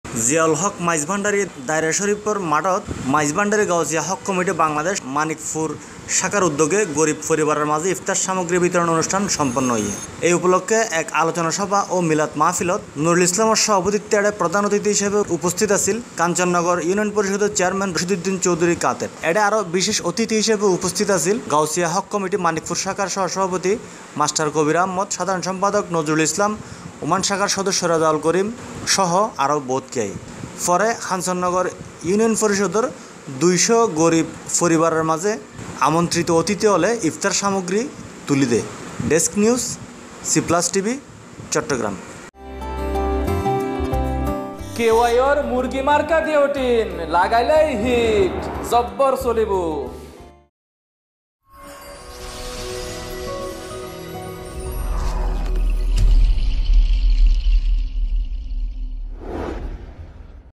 "মাইজভান্ডারী গাউছিয়া হক কমিটি "মানিকপুর শাখার উদ্যোগে ইফতার সামগ্রী বিতরন
"মাইজভান্ডারী গাউছিয়া হক কমিটি "মানিকপুর শাখার উদ্যোগে গরীব পরিবারের মাঝে ইফতার সামগ্রী বিতরন অনুষ্টান সম্পন্ন হয়েছে।ইফতার সামগ্রী বিতরন উপলক্ষে এক আলোচনা সভা ও মিলাদ মাহফিল গতকাল বিকাল ৫ টায় মানিকপুর শাখার সভাপতি মো: নুরুল ইসলামের সভাপতিত্ত্বে মানিকপুর বিশ্বঅলী শাহেনশাহ হযরত সৈয়দ জিয়াউল হক মাইজভান্ডারী দায়রা শরীফ প্রাঙ্গনে অনুষ্টিত হয়। এতে প্রধান অতিথি হিসেবে উপস্থিত ছিলেন কাঞ্চননগর ইউপি চেয়ারম্যান রশীদ উদ্দীন চৌধুরী কাতেব।এ সময় বিশেষ অতিথি হিসেবে উপস্থিত ছিলেন, গাউছিয়া হক কমিটি মানিকপুর শাখার সহ সভাপতি মাষ্টার কবির আহমদ, সাধারন সম্পাদক নজরুল ইসলাম, ওমান শাখার সদস্য রেজাউল করিম, ইউপি সদস্য আলহাজ্ব আবু তাহের, মো: আবছার উদ্দীন, মজাহারুল ইসলাম, মাওলানা মঈনুদ্দীন, দুবাই প্রবাসী আনোয়ার হোসেন, মনিরুল ইসলাম, মাষ্টার বদিউল আলম, রবিউল হক চৌধুরী মাসুদ, মঈন উদ্দীন আশেক ও আব্দুর রহিম।পরে কাঞ্চননগর ইউপির ২০০ গরীব পরিবারের মাঝে আমন্ত্রিত অতিথিরা ইফতার সামগ্রী বিতরন করেন।